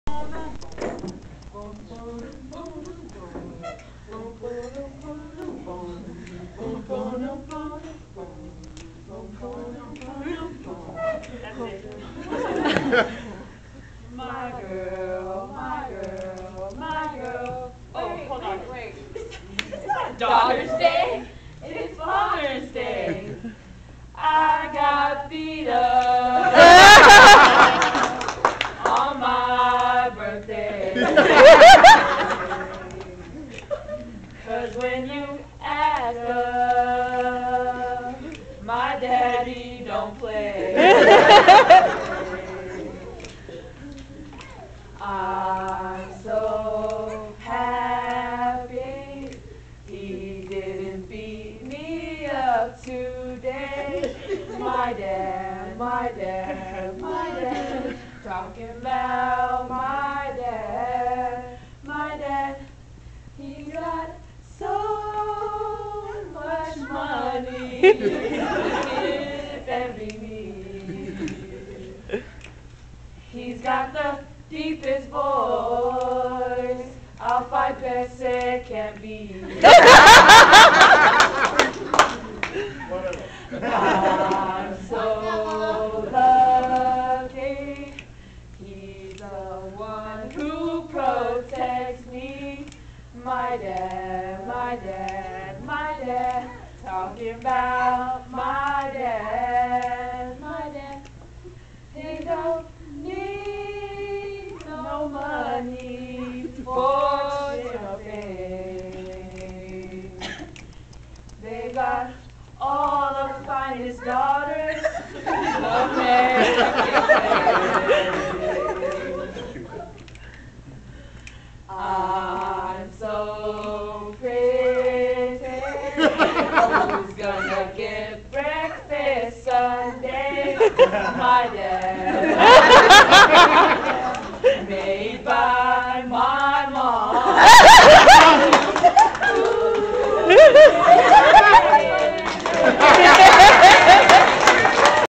Father's Day Tribute
the girls and their funny father's day song